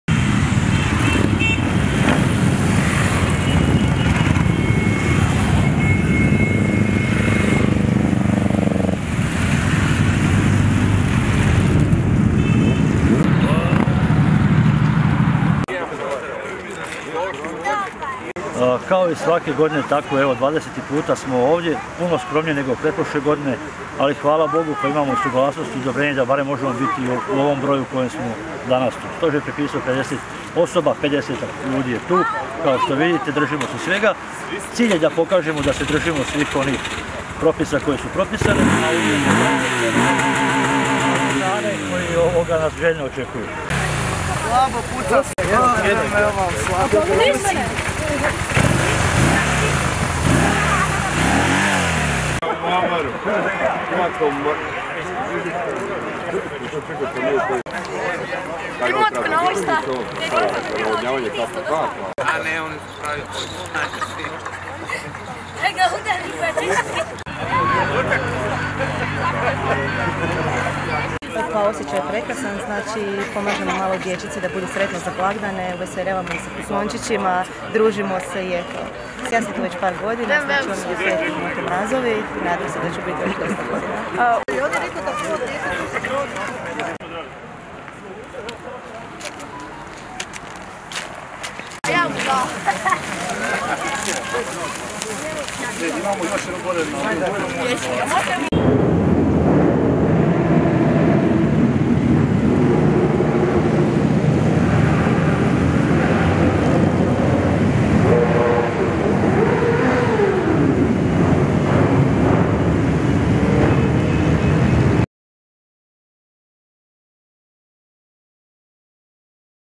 Motomrazovi provozali se ulicama Osijeka
Ove godine i to po 20. put Motomrazovi provozali su se ulicama Osijeka. Zbog mjera koje se tiću koronavirusa, broj Motomrazova znatno je smanjen a izostalo je i druženje s mališanima kojima su Motomrazovi donijeli bogate poklone. Ipak, provozavši se ulicama grada razveselili su ponajviše najmlađe.